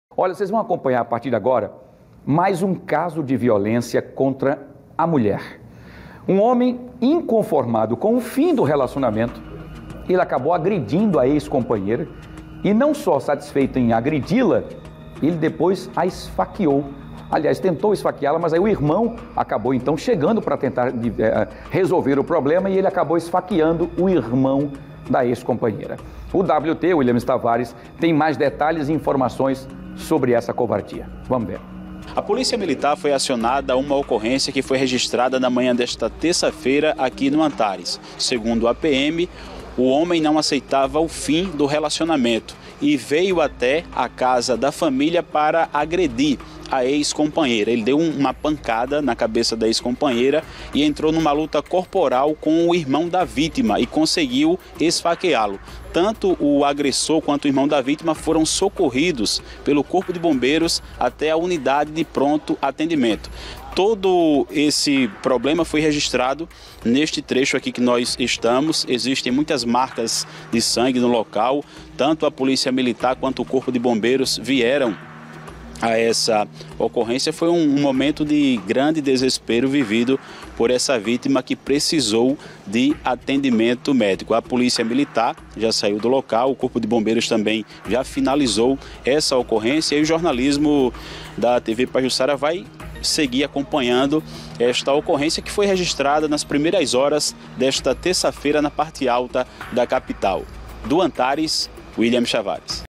Mulher é agredida e o irmão esfaqueado no Antares; agressor não aceita o fim da relação
Repórter Williamis Tavares traz mais detalhes sobre o caso.